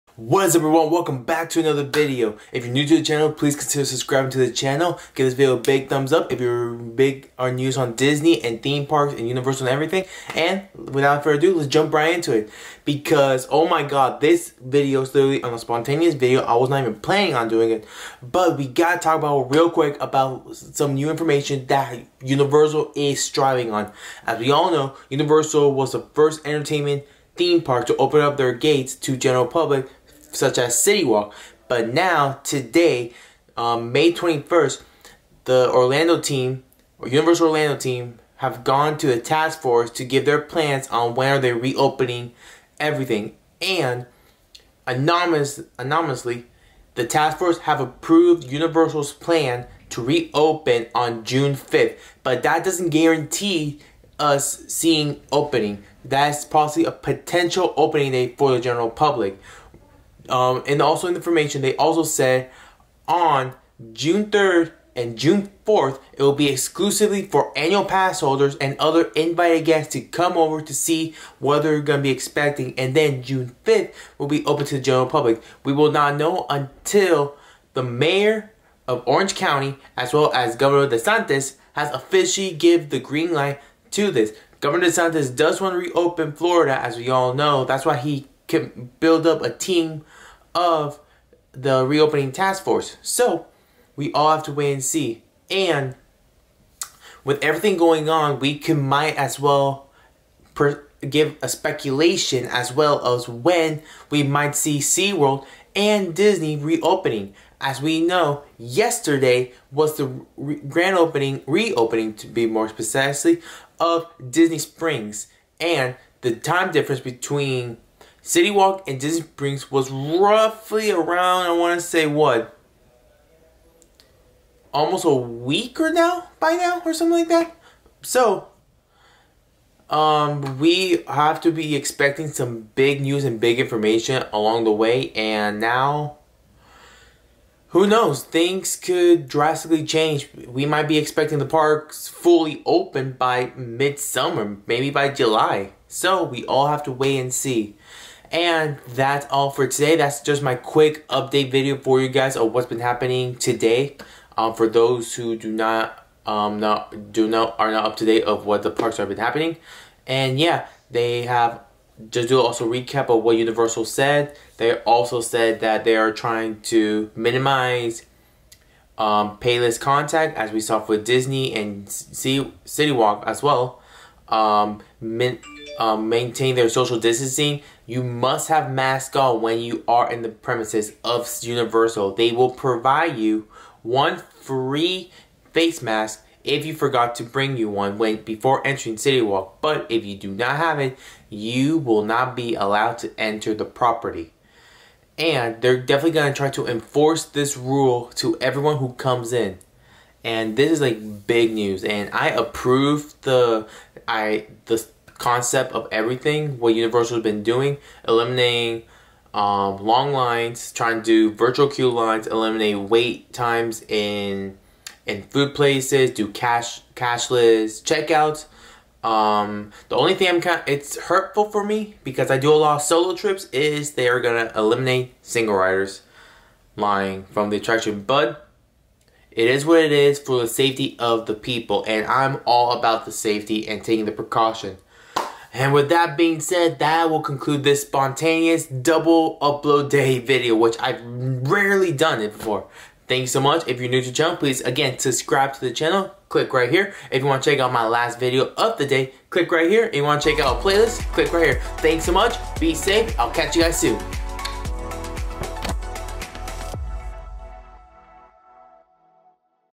BREAKING NEWS UNIVERSAL STUDIOS FLORIDA REOPENING VERY SOON |WHEN WILL UNIVERSAL STUDIOS REOPEN
Breaking news Universal it trying to have discussion of reopening of the park by June 5!! Hey everyone welcome back to the channel. There's been a lot of new info on whats been happening at the parks on when we are expecting on when they are probably opening to the public. At first it was my opinion but now we have HUGE NEWS on when we can expect on the opening of the parks.